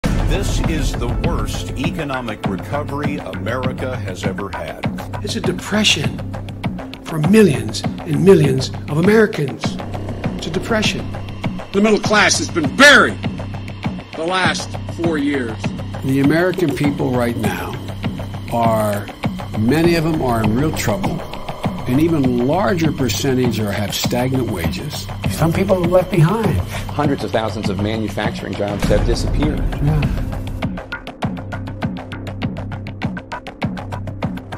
Joe Biden failed American workers and middle-class families!
When he was in charge, Joe Biden failed American workers and middle-class families.

Don't take our word for it. Take his!